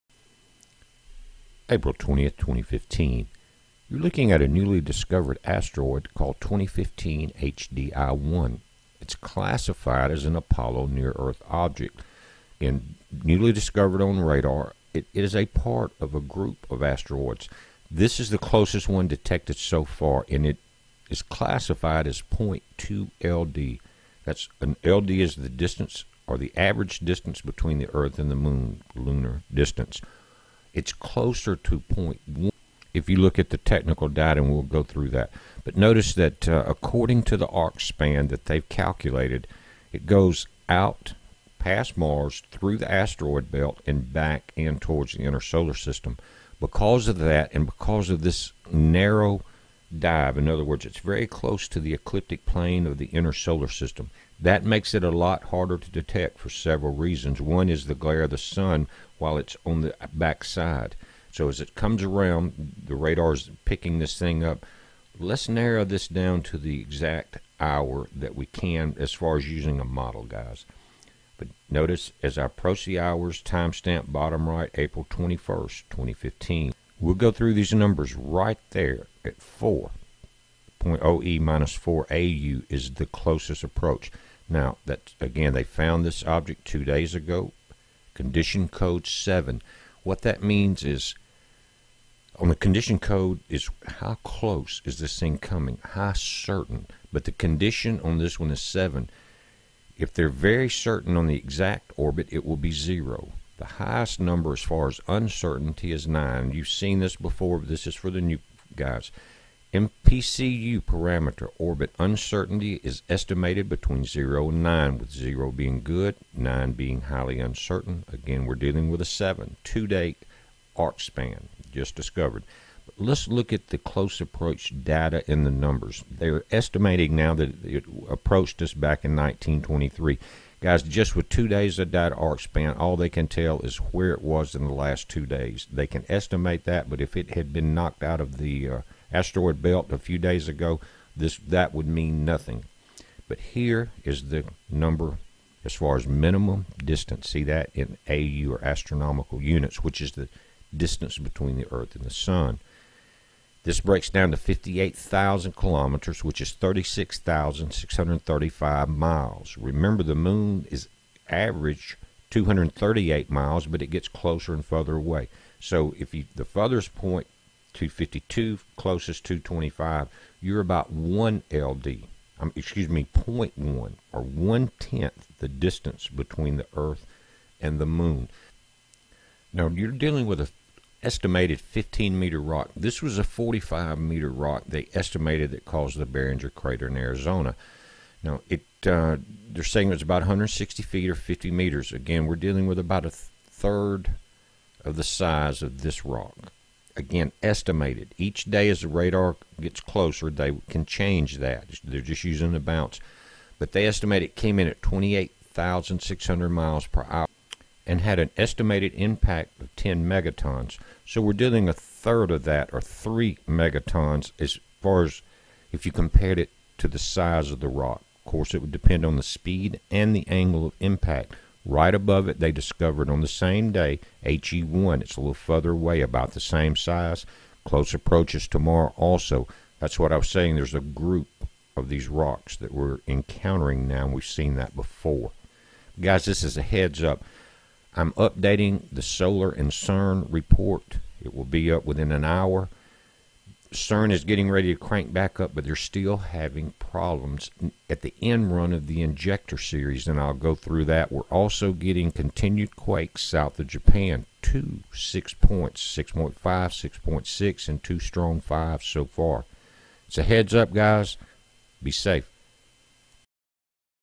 NEW ASTEROID CLOSE APPROACH
New Asteroid on its way.
Why are they so many of them lately?
Whats going on?